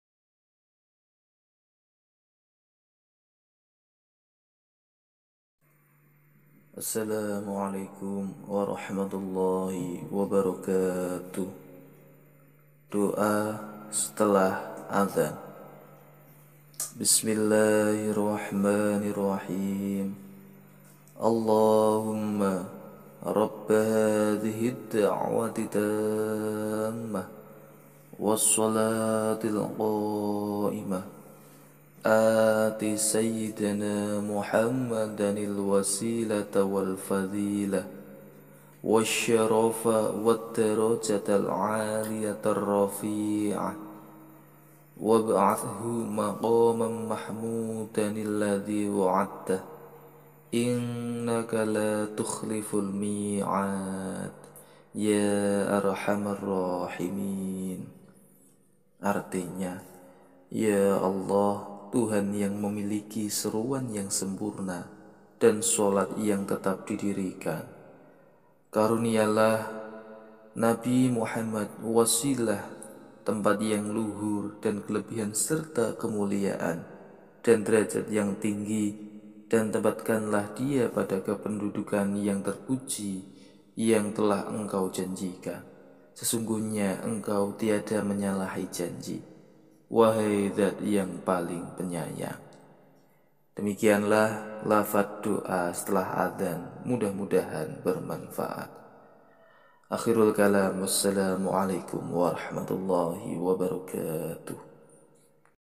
Doa Sesudah Adzan dan Artinya
اَللهُمَّ رَبَّ هذِهِ الدَّعْوَةِ التَّآمَّةِ، وَالصَّلاَةِ الْقَآئِمَةِ، آتِ مُحَمَّدَانِ الْوَسِيْلَةَ وَالْفَضِيْلَةَ وَالشَّرَفَ وَالدَّرَجَةَ الْعَالِيَةَ الرَّفِيْعَةَ وَابْعَثْهُ مَقَامًامَحْمُوْدَانِ الَّذِىْ وَعَدْتَهُ اِنَّكَ لاَتُخْلِفُ الْمِيْعَادَ يَآاَرْحَمَ الرَّحِمِيْنَ

Artinya :
Ya Allah Tuhan yang memiliki seruan yang sempurna dan shalat yang tetap didirikan, kurniailah Nabi Muhammad wasilah (tempat yang luhur) dan kelebihan serta kemuliaan dan derajat yang tinggi dan tempatkanlah dia pada kependudukan yang terpuji yang telah Engkaujanjikan, sesungguhnya Engkau tiada menyalahi janji, wahai dzat yang paling Penyayang.